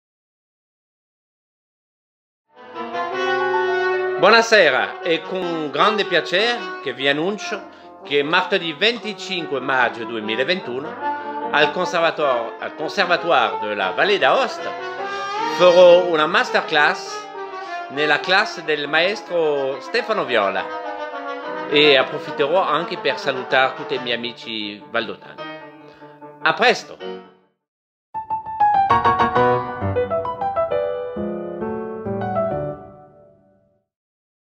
Masterclass per trombone di Vincent Lepape - Conservatoire de la Vallée d'Aoste
Si terrà martedì 25 maggio 2021 la Masterclass del M° Vincent Lepape primo trombone dell'orchestra del Teatro Regio Torino.

✅ INFO EVENTO
👉🏻 QUANDO: Martedì 25 maggio 2021
👉🏻 A CHE ORA: Dalle 10 alle 18
👉🏻 ANNO ACCADEMICO: 2020-21
👉🏻 DOVE: Auditorium del Conservatoire de la Vallée d'Aoste
👉🏻 CLASSE: Trombone del M' Stefano Viola